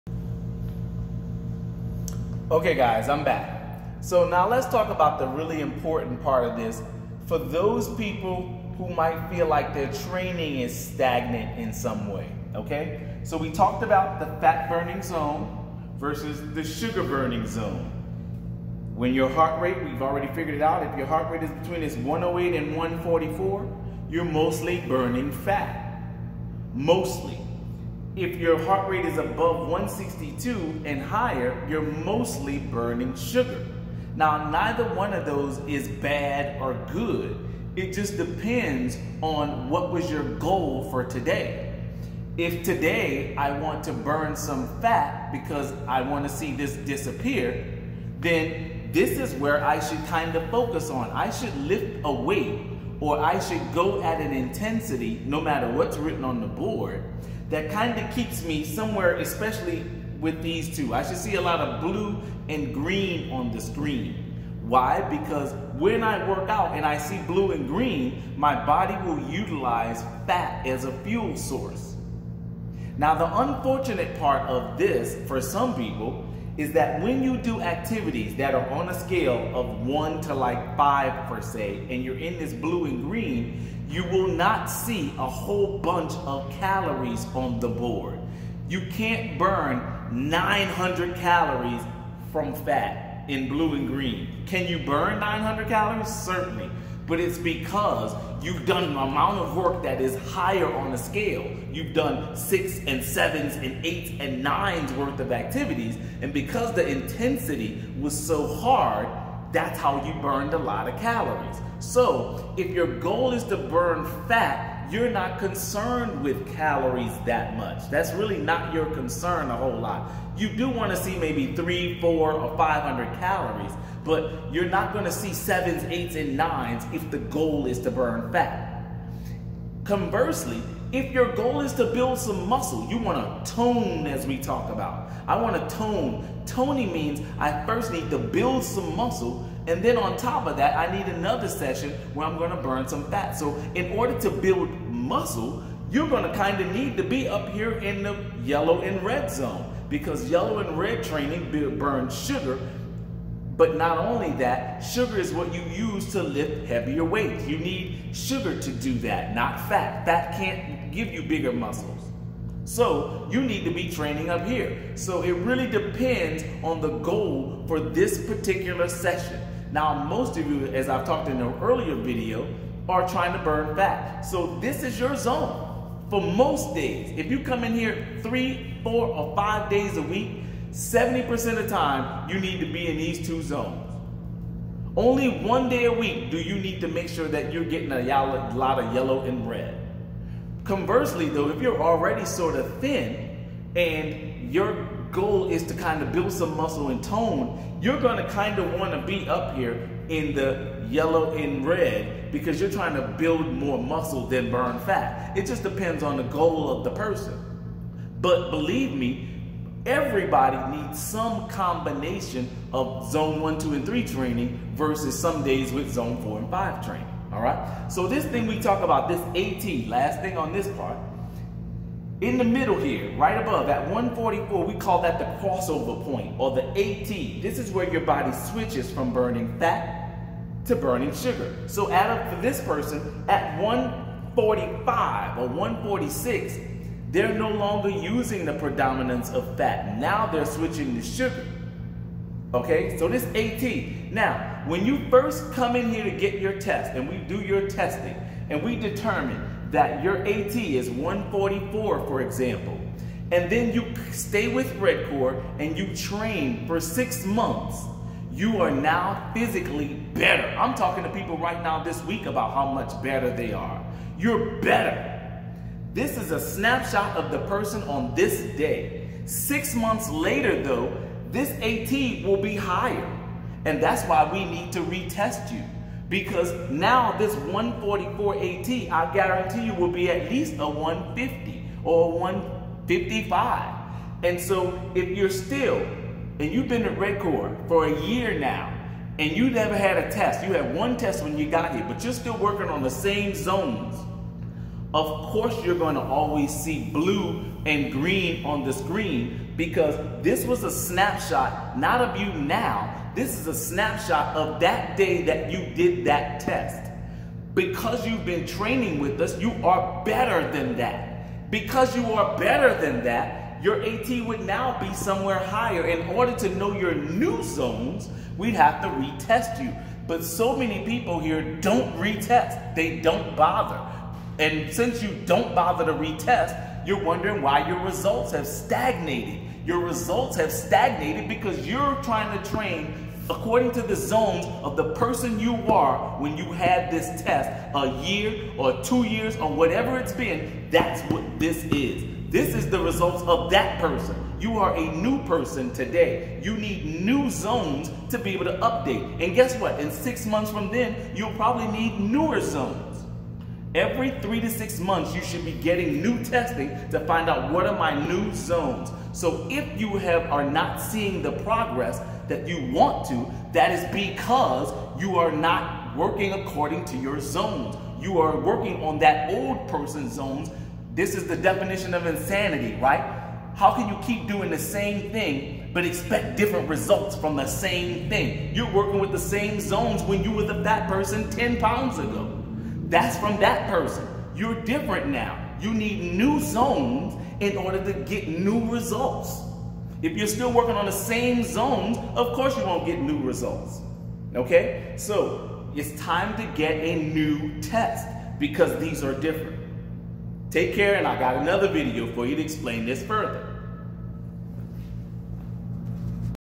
Understanding HR Training: Part 2
Anaerobic Threshold, Aerobic Threshold and what it means for you.